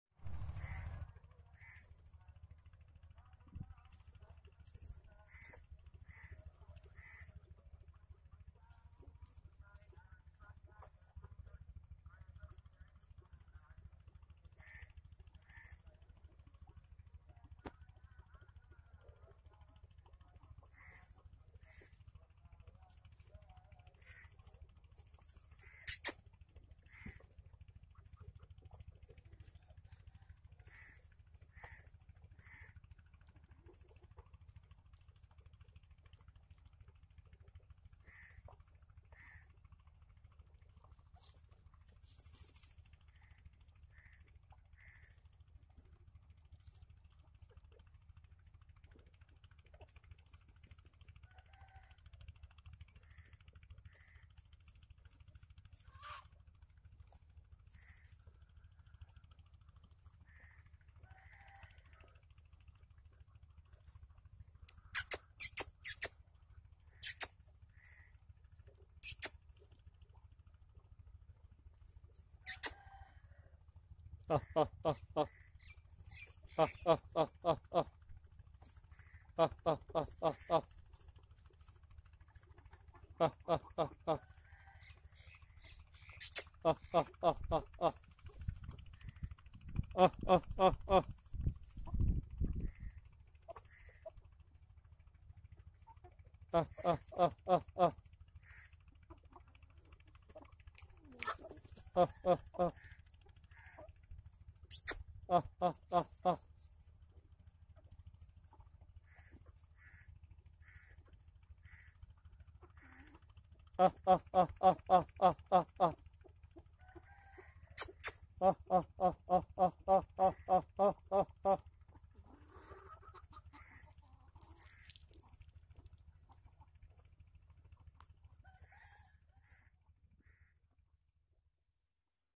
Beautiful wiew of Pakistani village lif.
Old asil lines preserved by Asgher Khan Rokhri and friends. We are trying to save Aseel blood line since 1960. This Aseel line was famous for its beauty and excellent fighting abilities. We don't promote birds fighting but we like to save some  ancient Aseel Blood lines. The name of this yellow breed is Miano walay peelay. This beautiful breed have very soft and shinny feathers  with very strong structure.